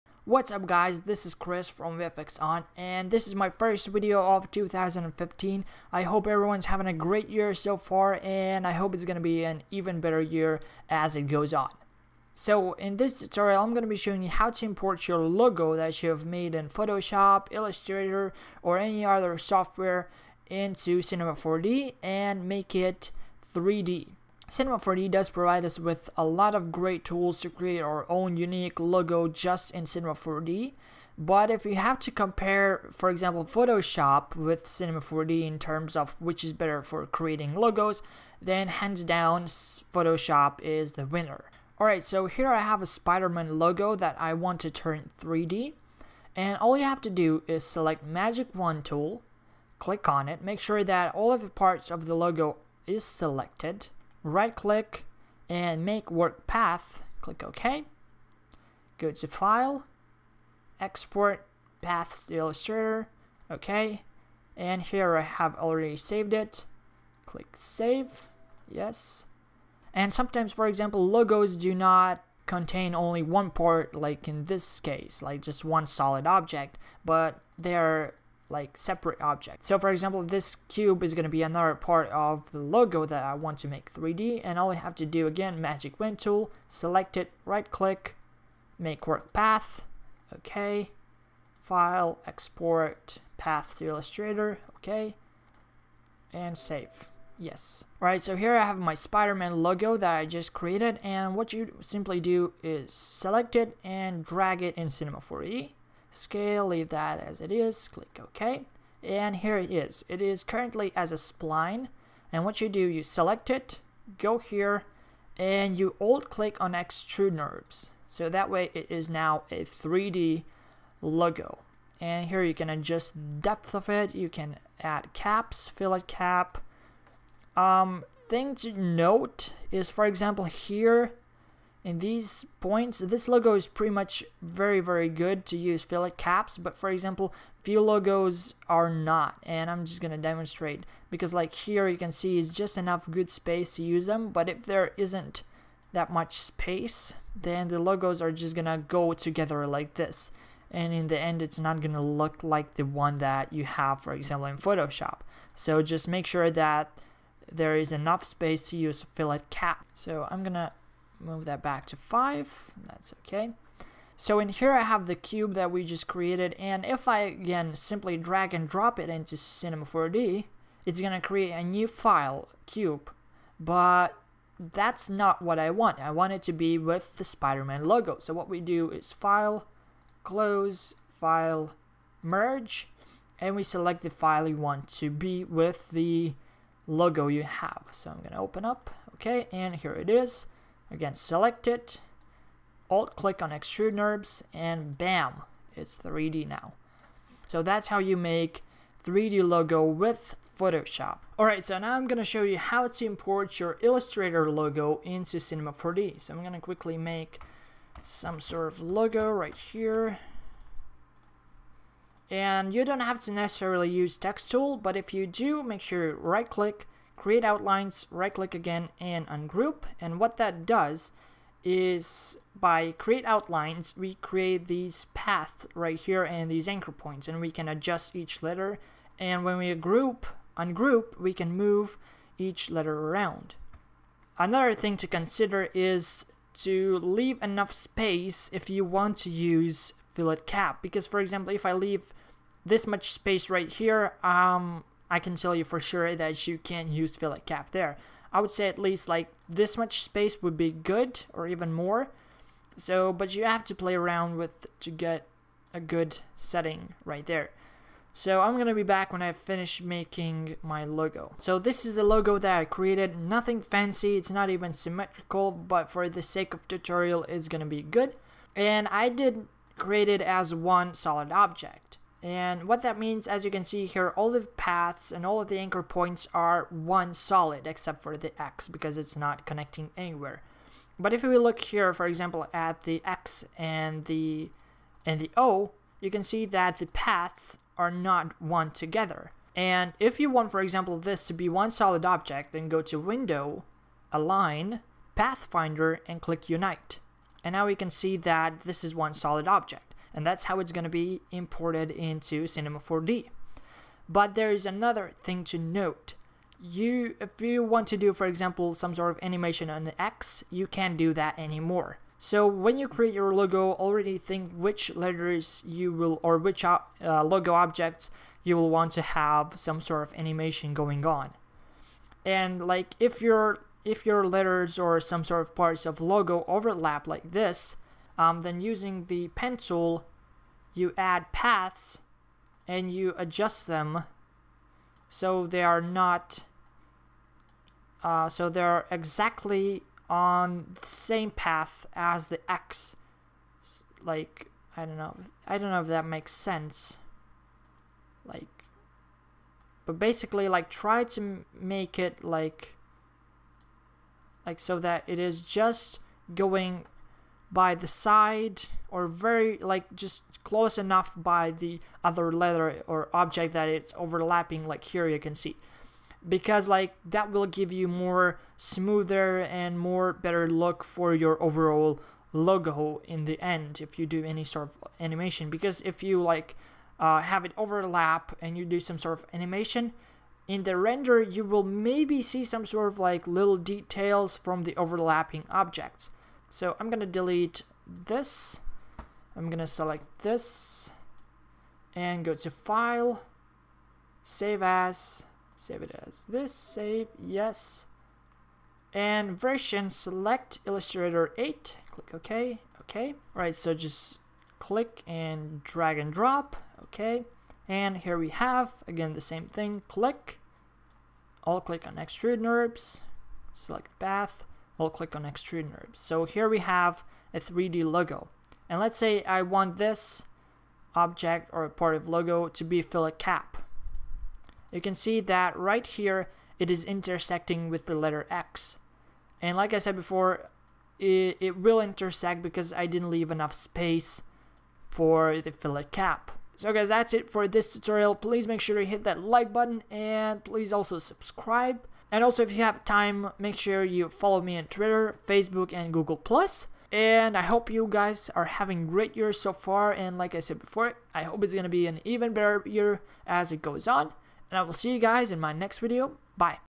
Photoshop, Illustrator, Cinema 4D Import Logo to C4D Tutorial
In this tutorial I am going to import logo from Photoshop & Illustrator into Cinema 4D!